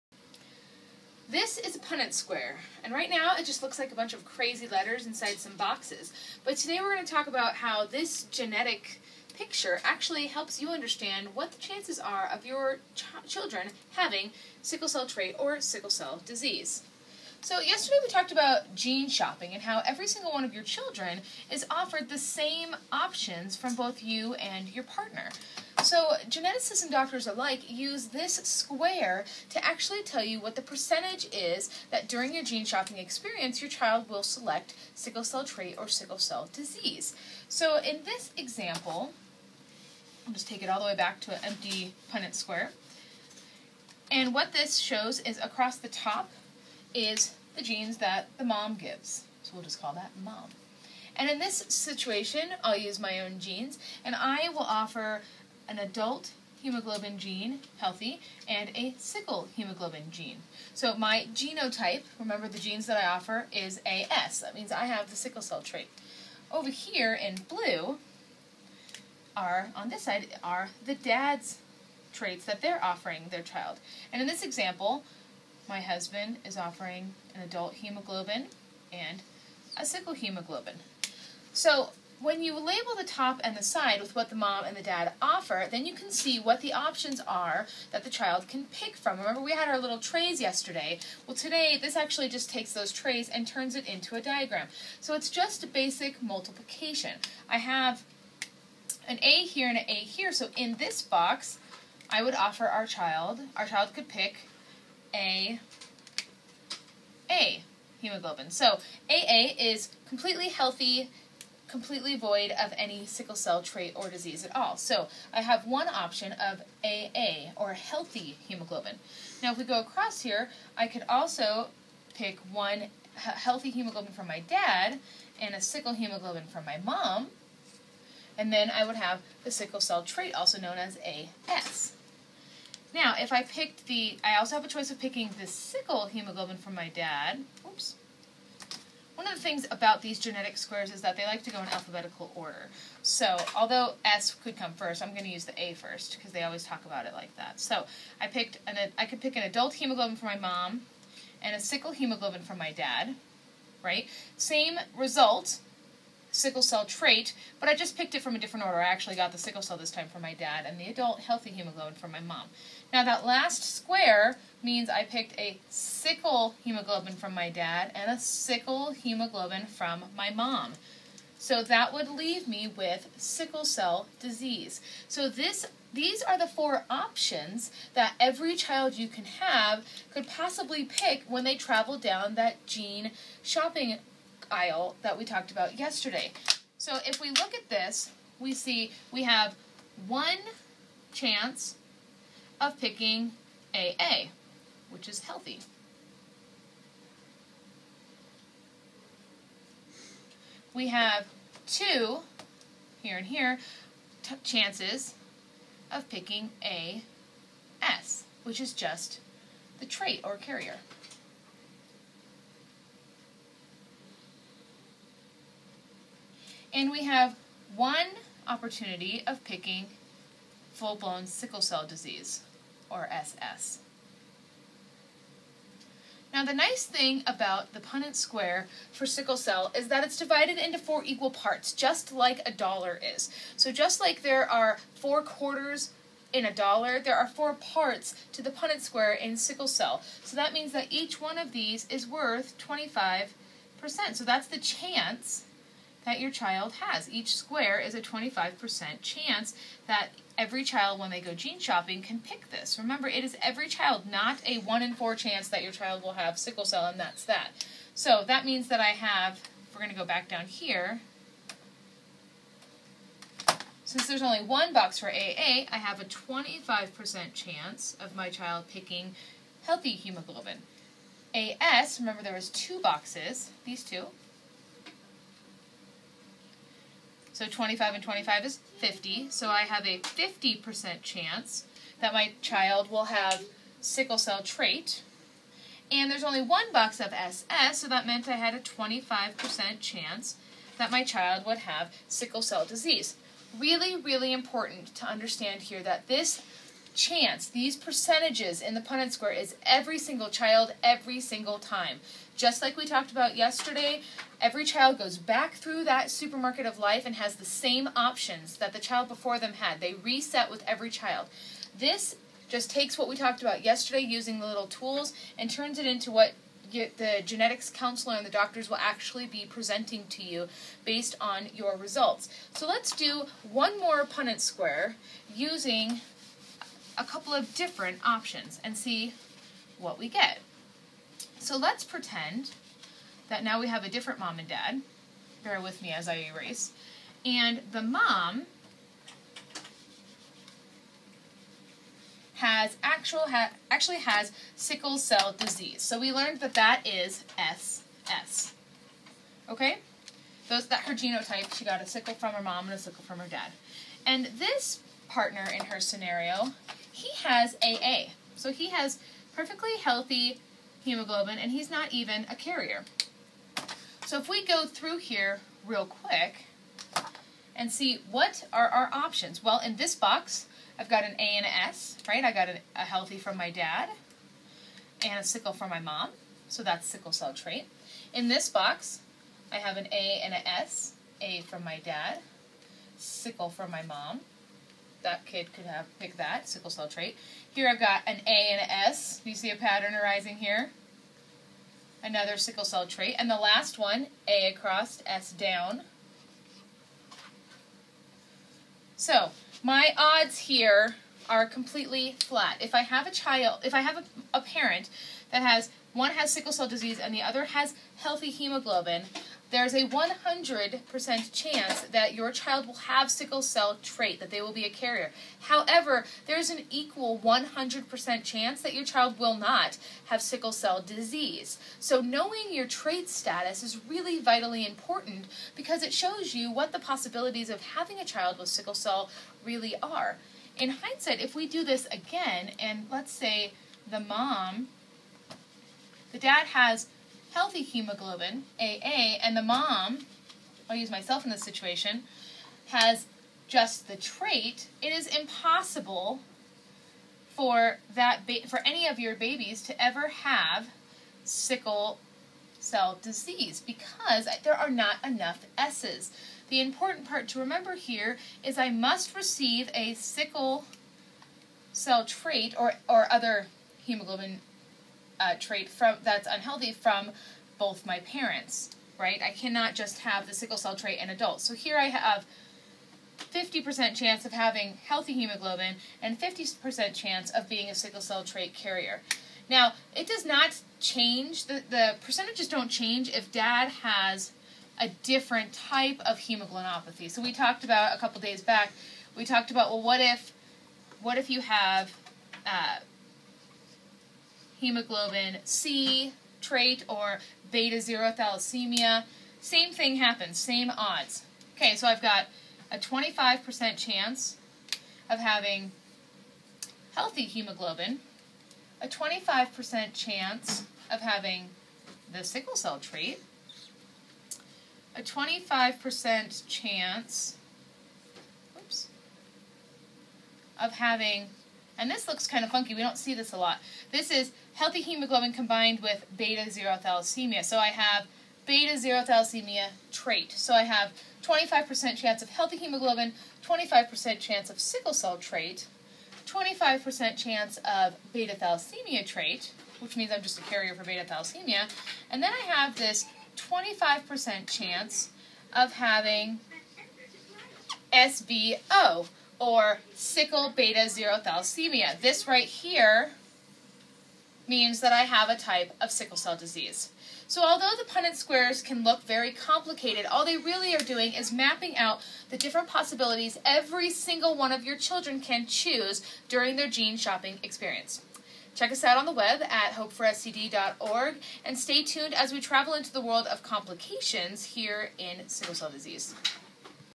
Punnett Square, No Pun Intended!
What are the chances that your children will have Sickle Cell? Well, it depends on what you're offering!